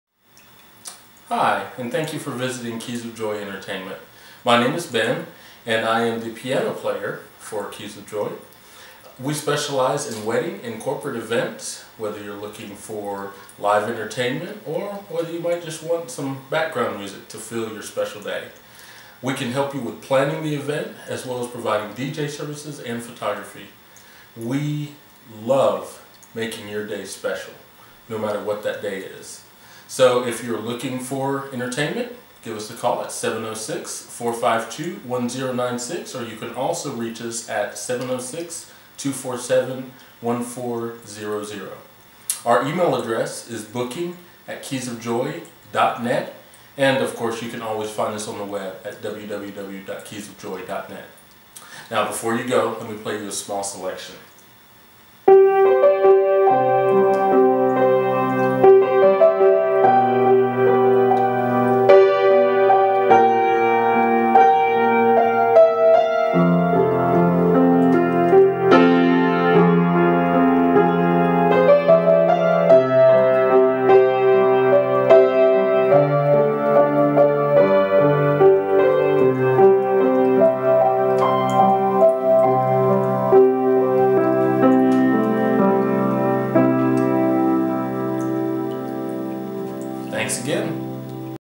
Keys of Joy Introduction & Promo
Welcome to Keys of Joy.  My name is Ben and I am glad you stopped by!!  We specialize in corporate and wedding entertainment.  We offer Acoustic Music, background music, DJ Services, Photography services, and professional Audio Engineering services.  We look forward to hearing from you!!

Thanks,

 Ben